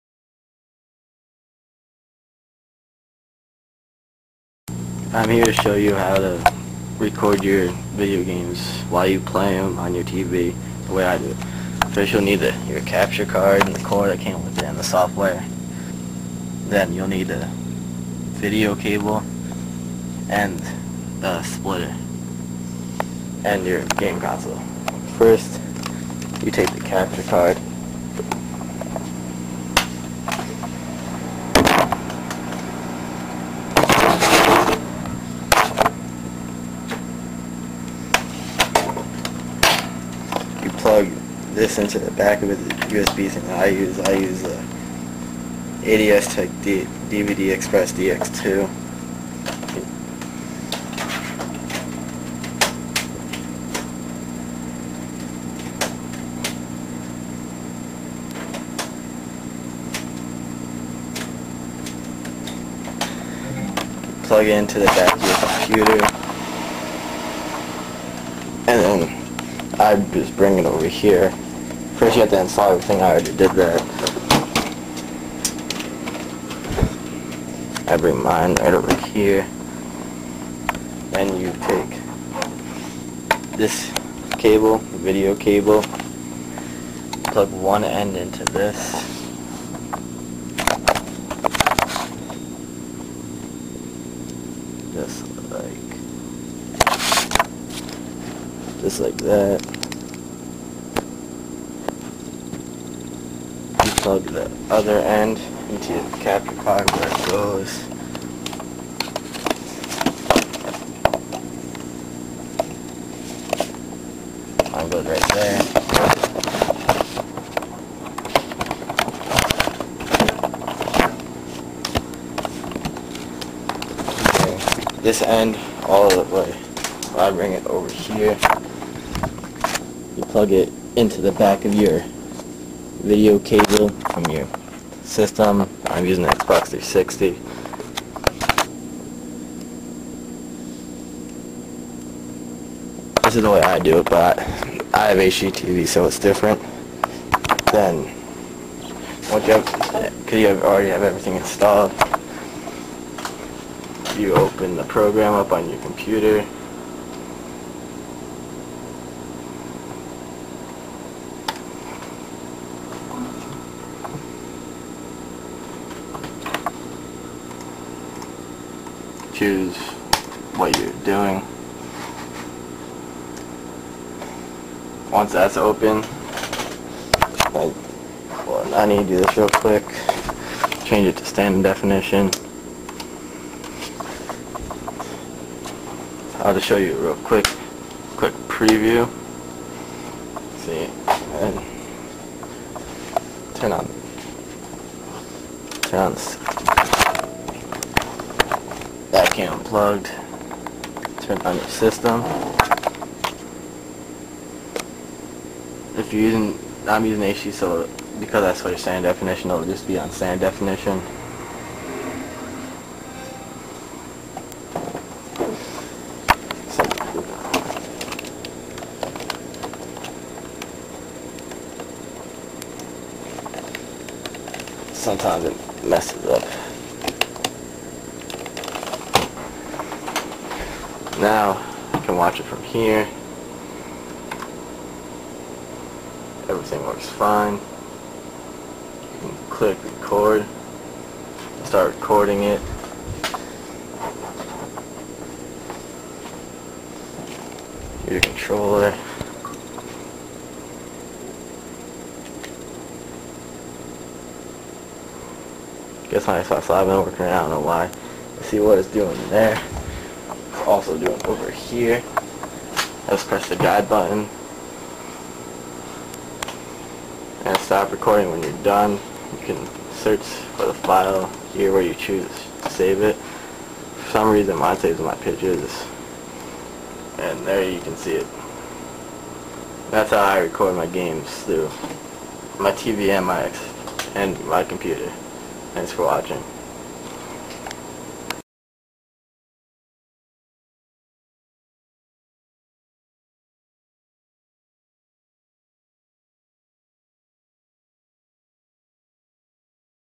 How to record video games tutorial
**Read Description!**

This is a video tutorial on how to record video games on to your computer while playing them on your tv at the same time.

**READ THIS**
I am using HD so i have a free SD video cord so i can use that but just play in SD on my tv while recoding in SD. You can't do this if you use SD.

**For HD Users*
Just Follow The Video

**For SD Users**
If you want to record video games while playing on your TV you will need to buy the S-Video Cable. When you buy that you plug the S-Video part into the back of the TV and Then plug the normal video cable into the capture card. I use to do that before i got HD.

HD=High Definition
SD-Standard Definition

My Set-up

26 inch Vizo 720p\1080i HDTV
Xbox 360 core (don't ask why) 120 gig. hard drive
5.1 Surround Sound